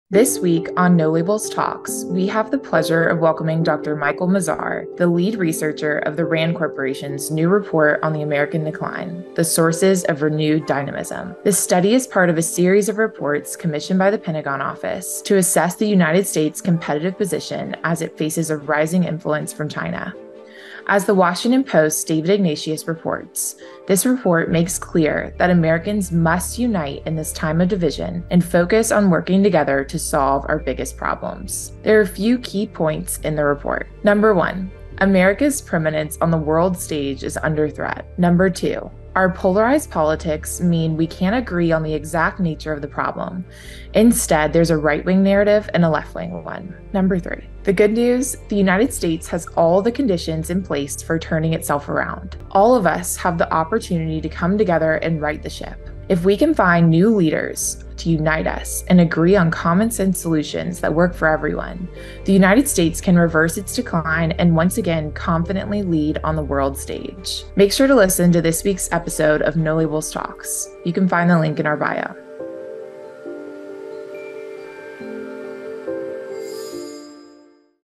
PREVIEW: No Labels Talks with Dr. Michael Mazarr
Join us for this week's NoLabelsTalks with Dr. Michael Mazarr, lead author of the RAND Corporation's important new report on American decline, “The Sources of Renewed Dynamism.”

Find No Labels on Facebook: Facebook.com/NoLabels
Check us out on Instagram